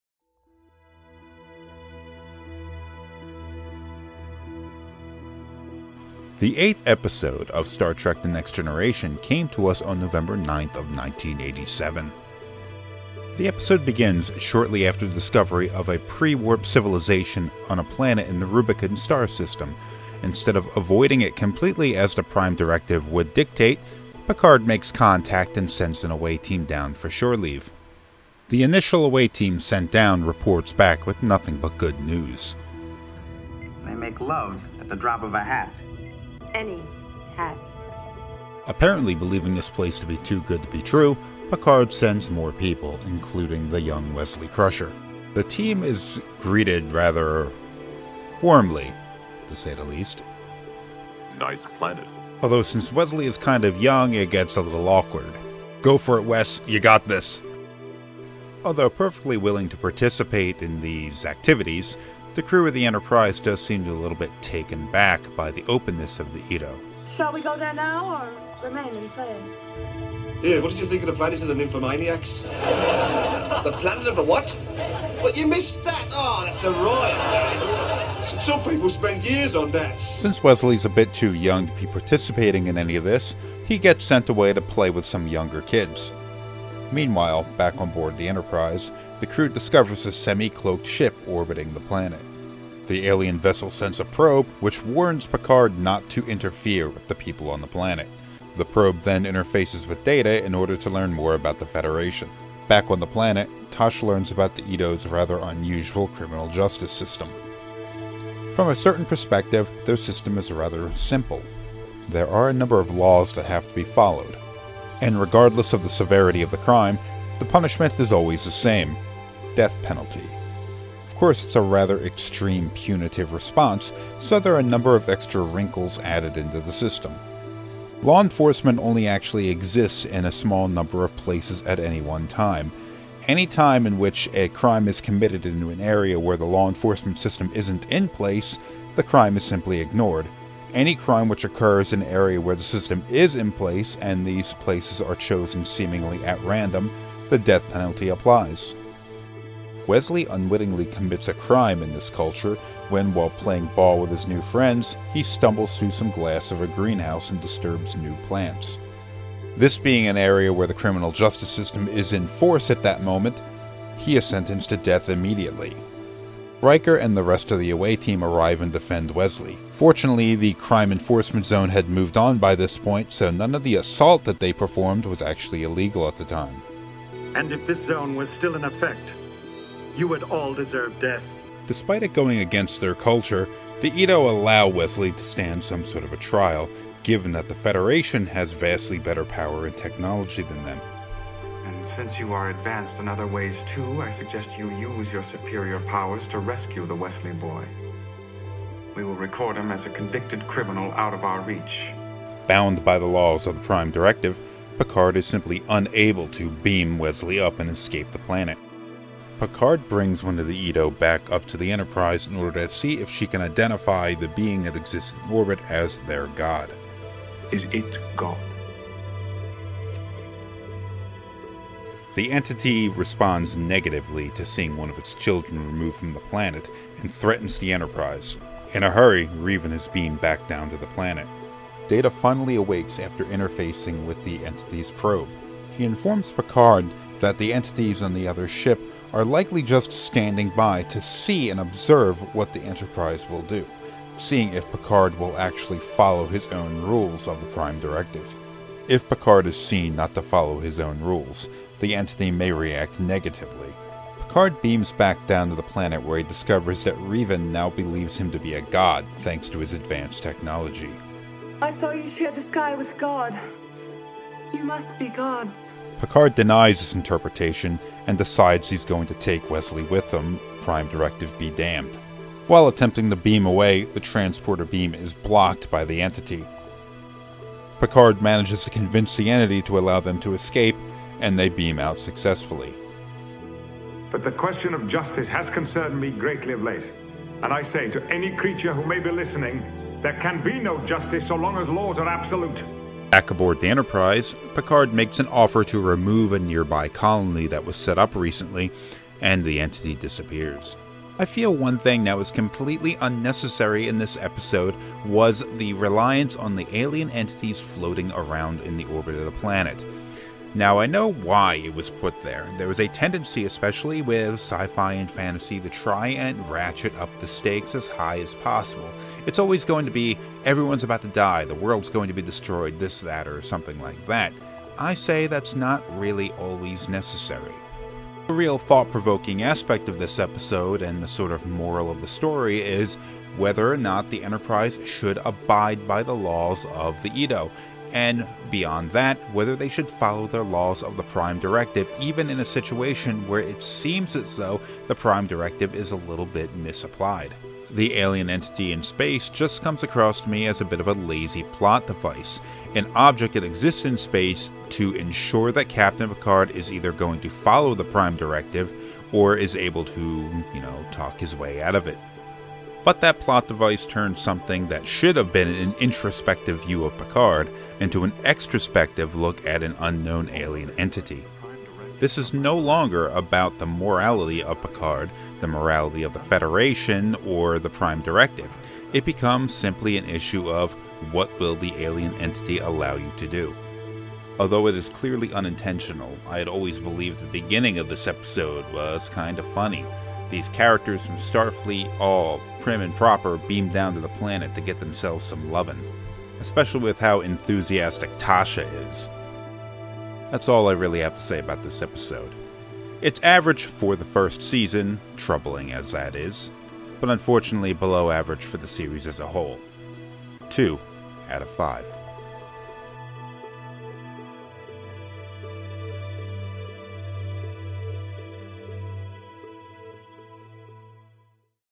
TNG Retro Review Episode 8: Justice
Jumping forward a couple of episodes, we hit Justice. Will these hit or miss first season episodes finally turn out something truly great? Probably not.